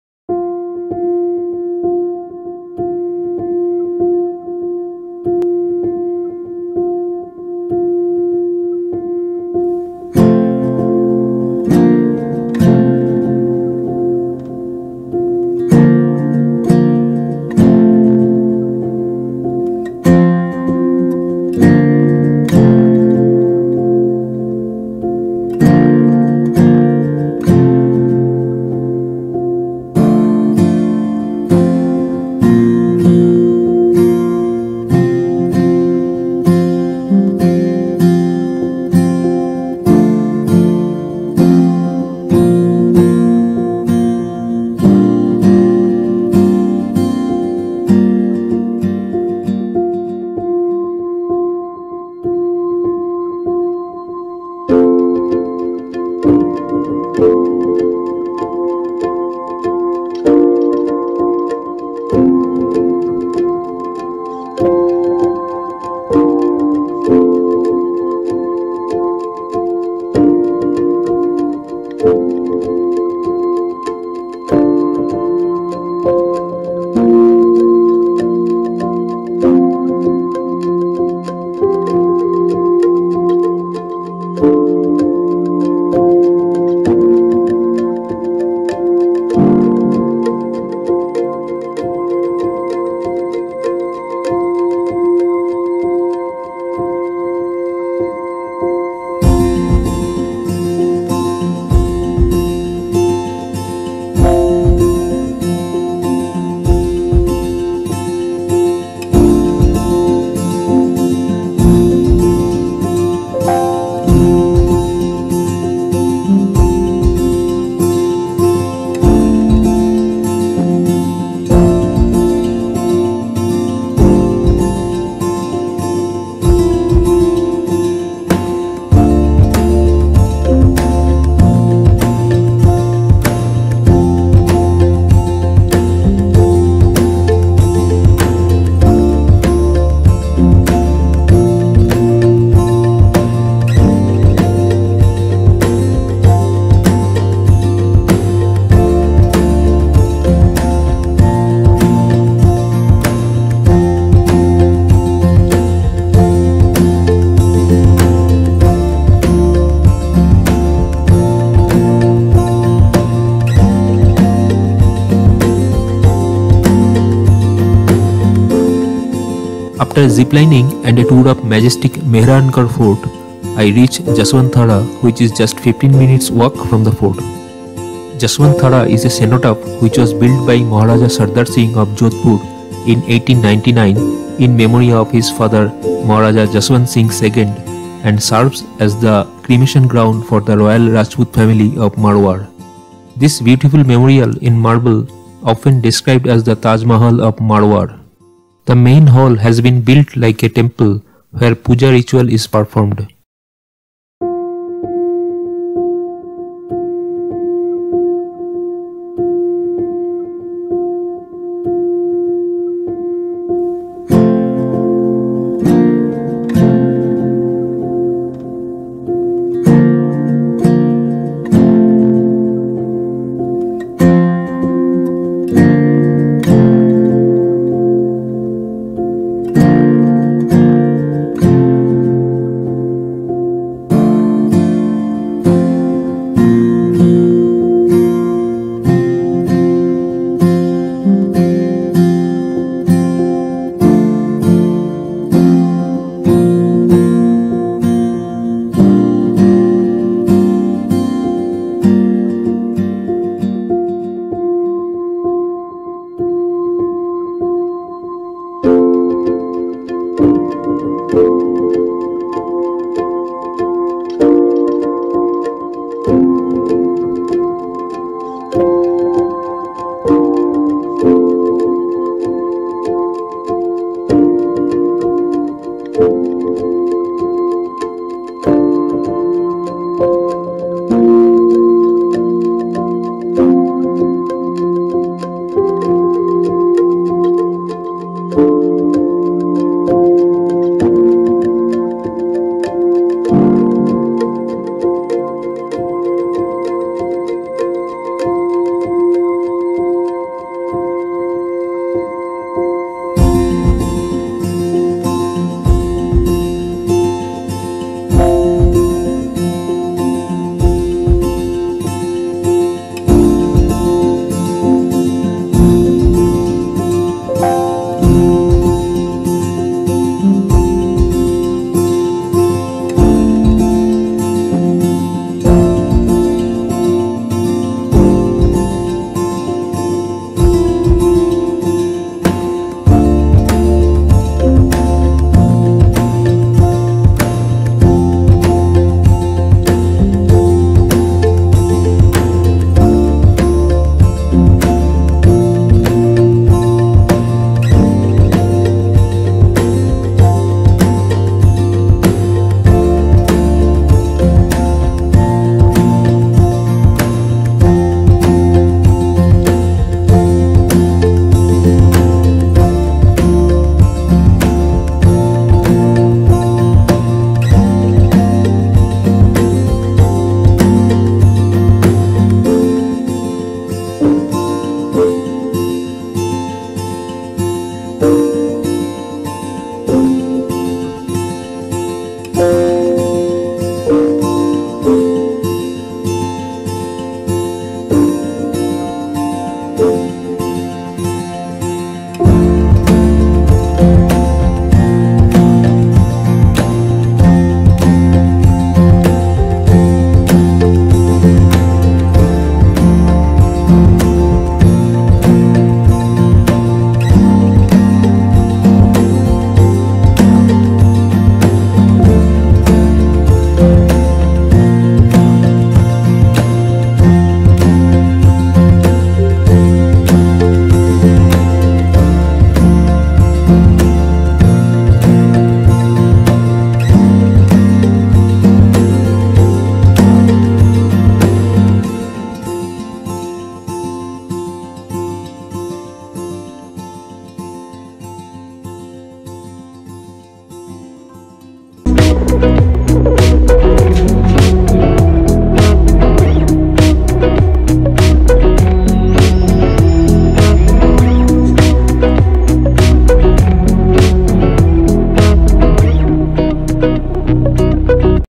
On The Way To Jaswant Thada | Taj Mahal Of Marwar | Rao Jodha Ji Statue
Rao Jodha Ji statue is set on a small hilltop along the road that leads to Jaswant Thada (near the Mehrangarh Fort). It was built in the honour of Rao Jodha Ji, who founded the beautiful city of Jodhpur.

Jaswant Thada, a beautiful memorial in marble, often described as the "Taj Mahal of Marwar" was built in memory of Maharaja Jaswant Singh Ji II of Jodhpur by his son Maharaja Sardar Singh Ji, completed in 1906.
The last rites of the rulers of Jodhpur were formerly performed at Mandore, the old capital of Marwar. After Maharaja Jaswant Singh Ji's demise in 1895 and the completion of this monument, they have been performed here. The smaller cenotaphs to east, mark the spots where his successors were cremated.
The main memorial has been built like a temple where puja ritual is performed and photographs of the deceased rulers are displayed. The worship of ancestors is common to the Rajput clans.
Dev Kund, the water body seen to the west of the complex, still used by the royal family for ritual bathing after cremation.

0:11  Rao Jodha Ji Statue
1:18  View of Mehrangarh Fort from Rao Jodha Ji Statue
3:24  Jaswant Thada
4:20  Cenotaph of Jaswant Singh Ji II
6:20  View of Mehrangarh Fort from Jaswant Thada
6:32  Royal Cenotaphs

Rajasthan videos -

Sri Lanka videos -

Kerala videos -